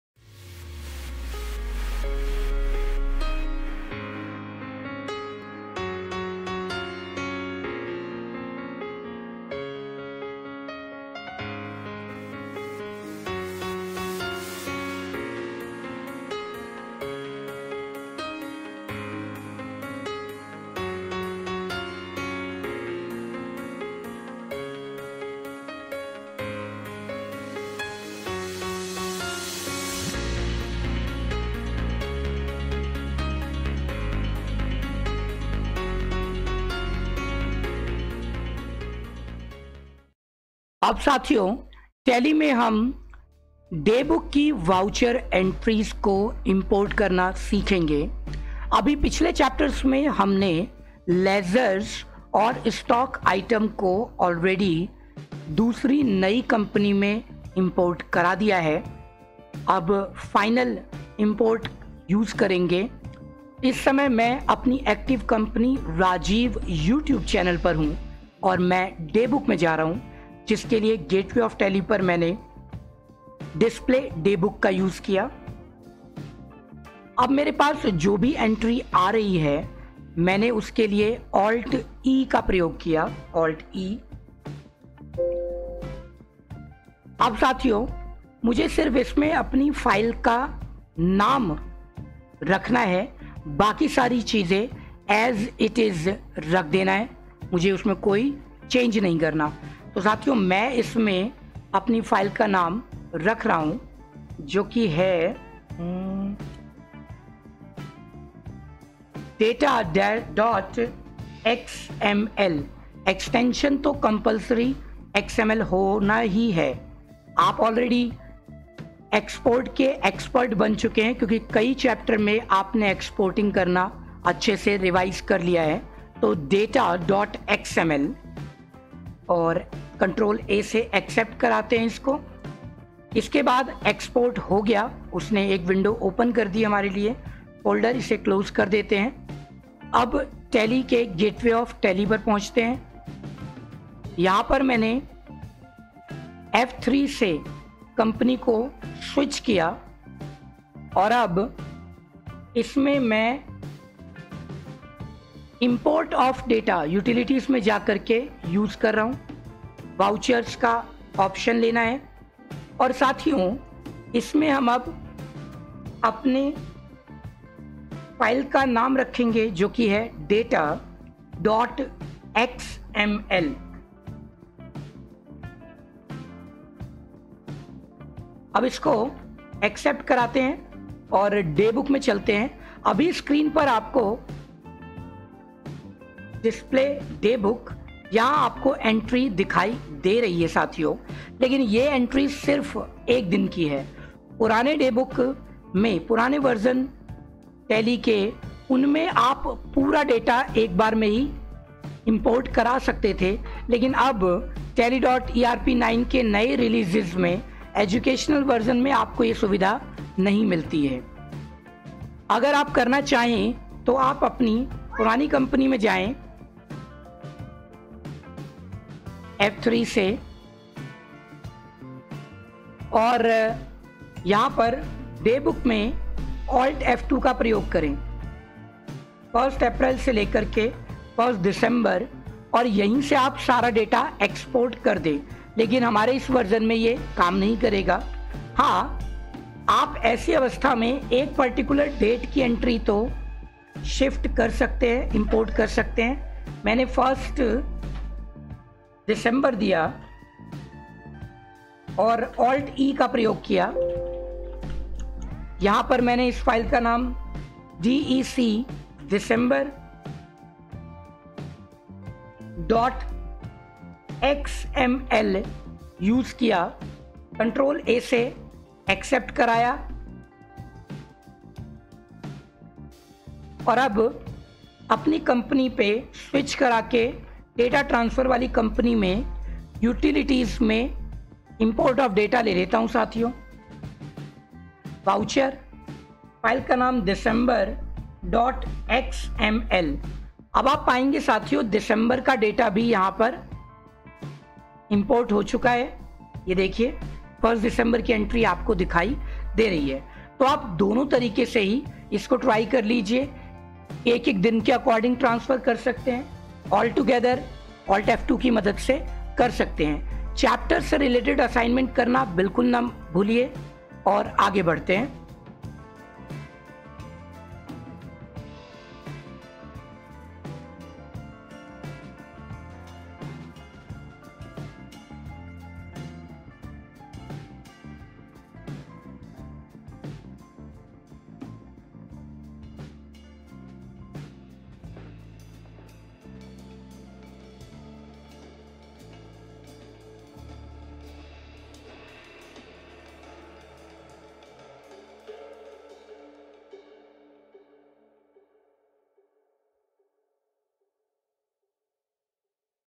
DayBook Voucher Import & Export in Tally ERP9 | Tally Chapter 33
Vouchers of Old Company to new company using Import/Export feature in Tally. DayBook Entries are possible to send in another company but we must send Ledgers and Stock First. We shared some new tricks & Tips in our solved Assignment Video. Do not Forget to watch them.

TALLY CHAPTER 33.
Assignment
Rajiv Mishra Youtube Channel
1.    State True or False.
1.1   We can Export DayBook Voucher Entries for a specific date .
1.2   We can Export DayBook Voucher Entries using CTRL E.
1.3   We must import Ledgers and Stock Before importing Vouchers.
1.4   Gateway of Tally shows the Last Voucher entered date.
1.5   We can not delete Daybook Voucher Entries.

2.    Multiple Choice Questions.
2.1  How do you know that a compny has Voucher Entries ?
A.   Through DayBook
B.   on Got, it Shows the last voucher entered date, if it has
C.   Statics can also show that

2.2   It shows total number of Voucher entries of voucher type.
A.   Account info
B.   Statics
C.   Voucher type
D.   B0th B & C

2.3  How will you  use to select specific entries in DayBook?
A.   CTRL A
B.   CTRL SHIFT Spacebar
C.    Spacebar

2.4 What is the key to select all entries of DayBook Screen ?
A.   CTRL A
C.    Spacebar

2.5  What is the key to change Period.?
A.   F2
B.   ALT F2
C.   CTRL F2
D.   Both A & B

3.    Write the answer of these questions
3.1  Create 10 New Voucher Entries of Sales which has different
       Item each.
3.2  How will you create Duplicate Voucher entries.

Practical Task

1 Revise the previous chapters.

Enjoy Learning!!!
Best Wishes